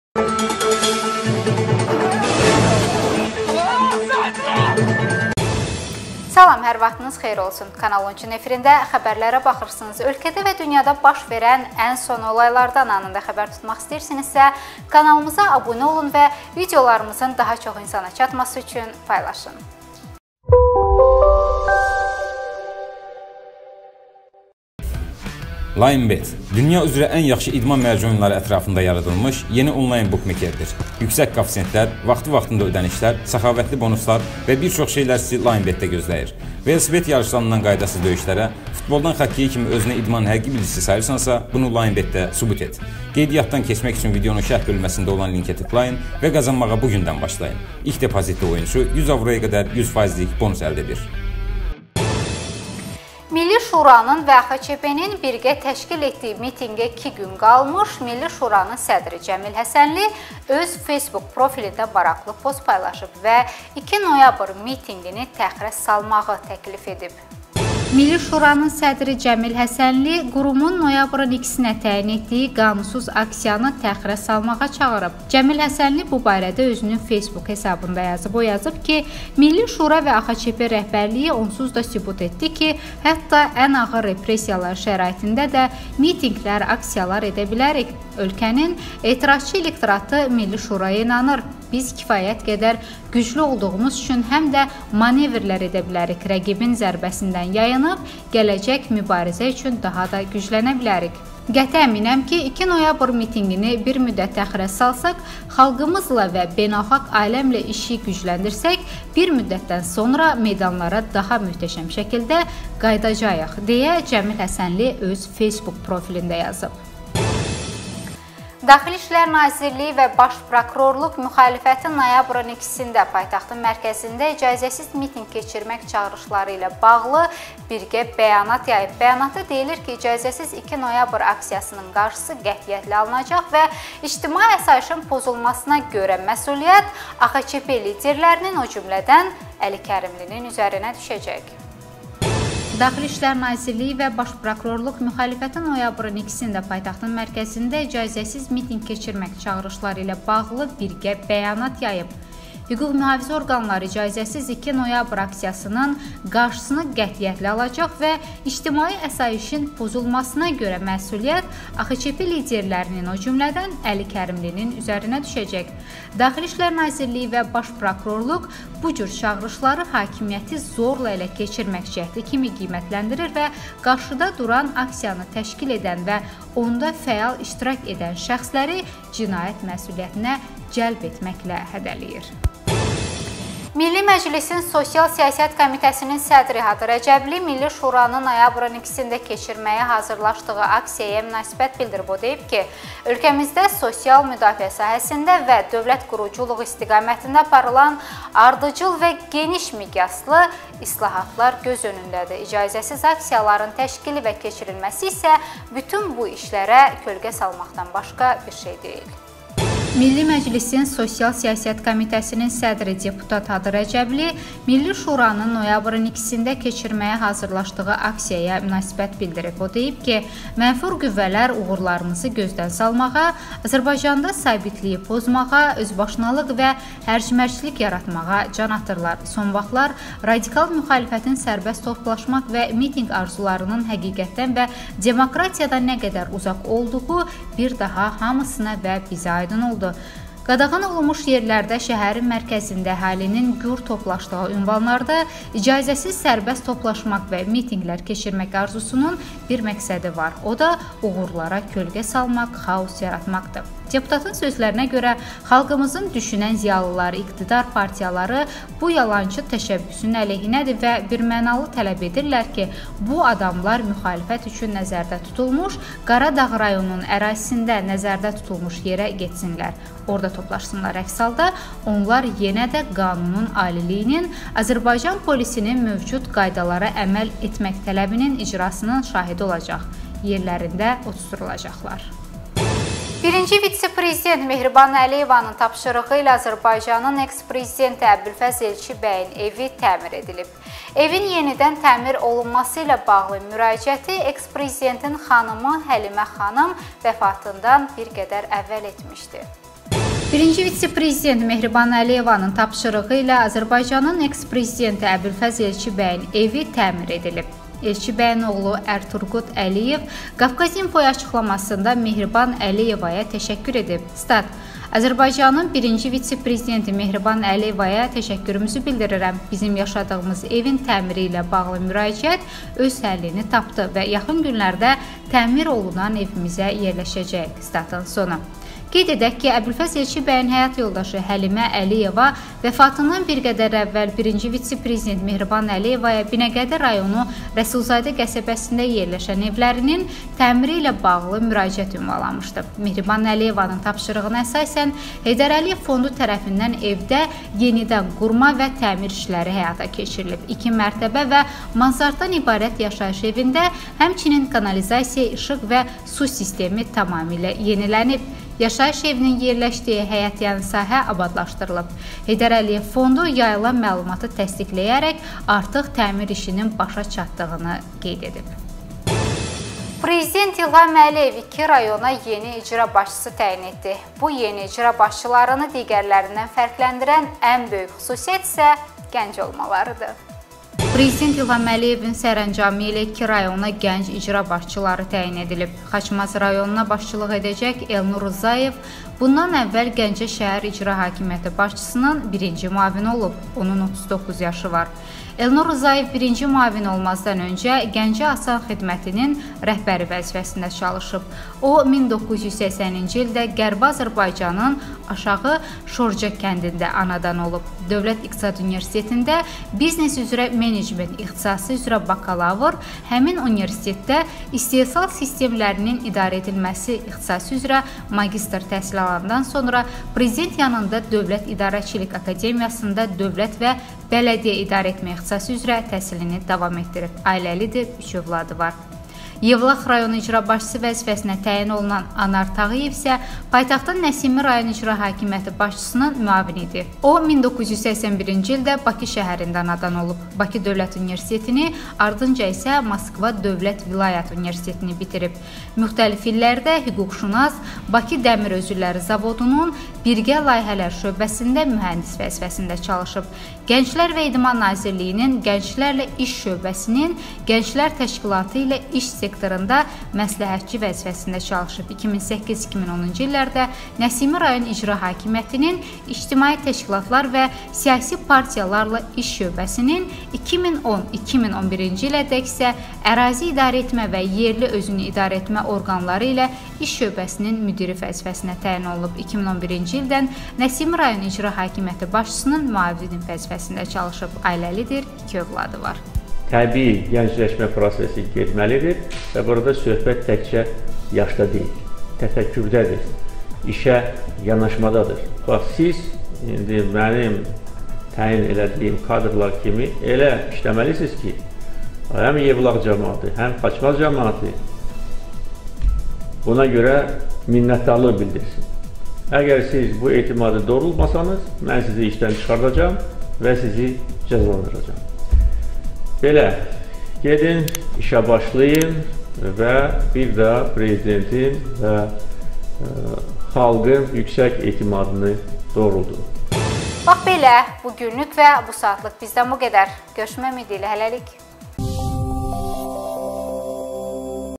2 noyabr mitinqi təxirə salındı; "Toplaşmaq azadlığı özbaşınalıq demək deyil"-Hadi Rəcəbli
Bu linkə tıklayıb oynayın və qazanın:
Xəbərlərə baxırsınız.Milli şuranın və AXCp-nin birgə təşkil etdiyi mitinqə iki gün qalmış  Milli şuranın sədri Cəmil həsənli öz facebook profilində maraqlı post paylaşıb və 2 noyabr mitinqini təxirə salmağı təklif edib.

DİN və Baş Prokurorluq müxalifətin noyabrın 2-də paytaxtın mərkəzində icazəsiz mitinq keçirmək çağırışları ilə bağlı birgə bəyanat yayıb.Bəyanatda deyilir ki, icazəsiz 2 noyabr aksiyasının qarşısını qətiyyətlə alacaq və ictimai asayişin pozulmasına görə məsuliyyət AXCP liderlərinin, o cümlədən Əli Kərimlinin üzərinə düşəcək.

 Milli Məclisin Sosial siyasət komitəsinin sədri Hadi Rəcəbli Milli Şuranın noyabrın 2-də keçirməyə hazırlaşdığı aksiyaya münasibət bildirib .O deyib ki, Ölkəmizdə sosial müdafiə sahəsində və dövlət quruculuğu istiqamətində aparılan ardıcıl və geniş miqyaslı islahatlar göz önündədir.İcazəsiz aksiyaların təşkili və keçirilməsi isə bütün bu  işlərə kölkə salmaqdan başqa bir şey deyil.

Birinci Vitse-Prezident Mehriban Əliyevanın tapşırığı ilə Azərbaycanın eks-prezidenti Əbülfəz Elçibəyin evi təmir edilib. Evin yenidən təmir olunması ilə bağlı müraciəti eks prezidentin xanımı Həlimə xanım vəfatından bir qədər əvvəl etmişdi

Preziden İlham Əliyev iki rayona yeni icra başcısı təyin etdi.bu yeni icra başcılarını digərlərindən fərqləndirən ən böyük xüsüsiyyət isə gənc olmalarıdır.